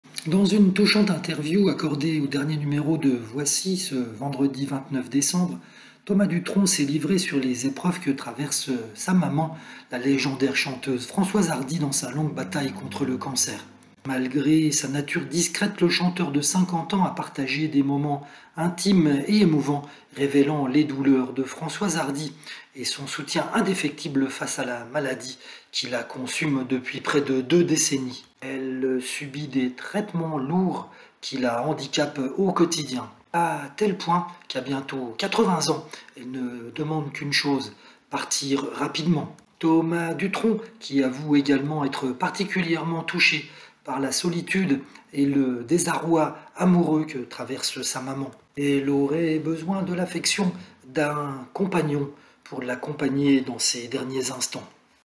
"Un compagnon pour lui fermer les yeux" : révélations sur la solitude affective de Françoise Hardy
Thomas Dutronc ému par "la solitude et le désarroi amoureux" de sa mère : ses touchantes confidences sur Françoise Hardy atteinte par un cancer à 80 ans